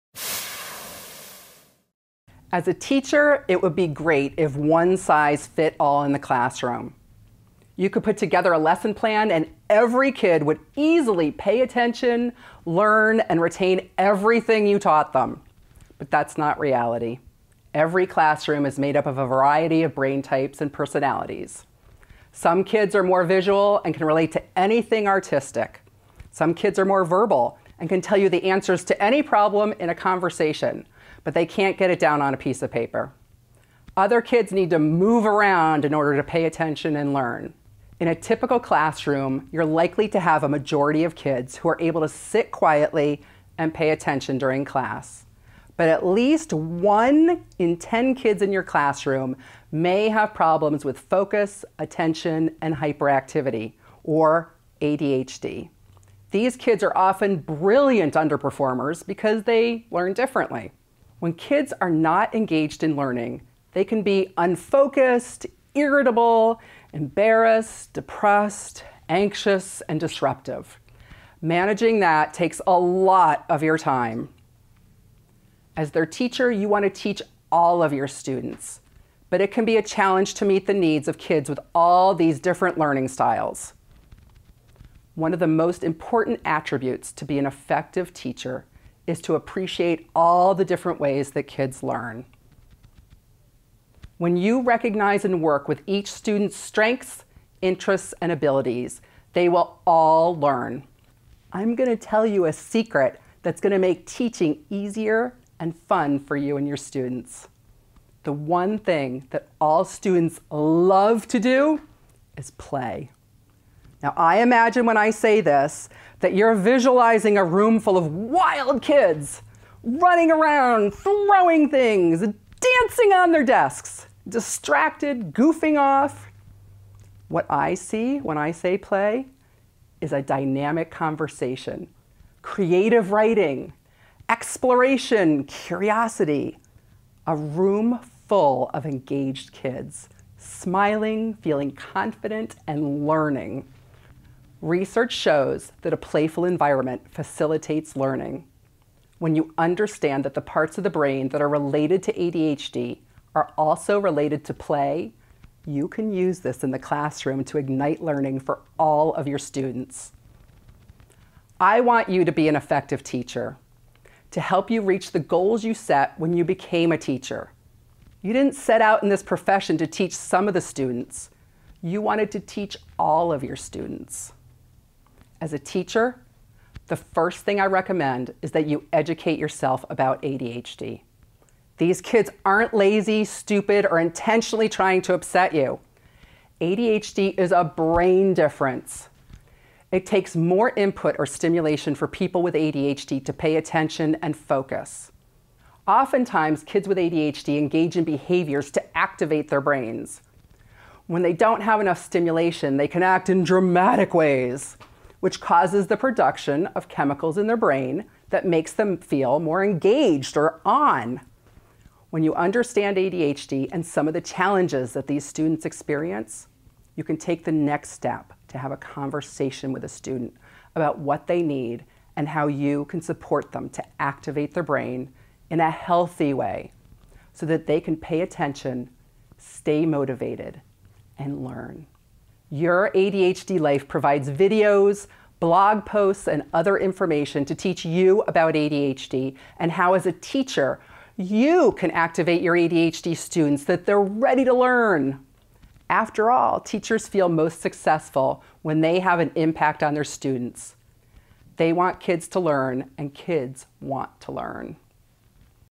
How to Make Teaching Easy and Fun *
As a teacher it would be great if one size fit all in the classroom….you could put together a lesson plan and every kid would easily pay attention, learn and retain everything you taught them. But that’s not reality..Every classroom is made up of a variety of brain types and personalities.  some kids are more visual and can relate to anything artistic...some kids are more verbal and can tell  you the answers to any problem in a conversation but can’t get it down on paper. Other kids need to move around in order to pay attention and learn.

In a typical classroom you are likely to have a majority of kids who are able to sit quietly and pay attention during class.  But at least one in 10 kids in your class may have problems with focus attention and hyperactivity- or ADHD.  These kids are often brilliant underperformers because they learn differently.

When kids are not engaged in learning they can be unfocused, irritable, embarrassed, depressed, anxious, and disruptive.  Managing that takes a lot of your time!

As their teacher..you want to teach all of your students. But it can be a challenge to meet the needs of kids with all these different learning styles.

One of the most important attributes to be effective an effective teacher is to appreciate all the different ways kids learn.

When you recognize and work with each student’s strengths, interests and abilities they will all learn.

I’m going to tell you a secret that will make teaching easier and fun for you and your students.  The one thing that all students love to do is  play --.
 I imagine when I say this that you are visualizing a room full of wild kids running around, throwing things, dancing on their desks, distracted and goofing off.  What I see when I say play is a dynamic conversation, creative writing, exploration, curiosity-   a room full of engaged kids.  Smiling, feeling confident and learning.

Research shows that  a playful environment facilitates learning. When you understand that the parts of the brain that are related to ADHD are also related to play you can use this in the classroom to ignite learning for all of your students.

I want you be an effective teacher. To help you reach the goals you set when you became a teacher.  You didn’t set out in this profession to teach some of the students, you want to teach all of your students.

As a teacher, the first thing I recommend is that you educate yourself about ADHD ...these kids aren’t lazy, stupid or intentionally trying to upset you. ADHD is a brain difference. It takes more input or stimulation for people with ADHD to pay attention and focus.

Often times kids with ADHD engage in behaviors to activate their brains.  When they do not have enough stimulation they can act in dramatic ways -- which causes the production of chemicals in their brain that makes them feel more engaged and “on.”   When you understand ADHD and some of the challenges these students experience, you can take the next step to have a conversation with these students about what they need and how you can support them to activate their brains in a healthy way so they can pay attention, stay motivated and get things done.

YAL provides videos, blog posts and other information to teach you about ADHD and how as a teacher you can activate your ADHD students so they are ready to learn.  After all, Teachers feel most successful when they have an impact on their students, they want kids to learn and kids want to learn.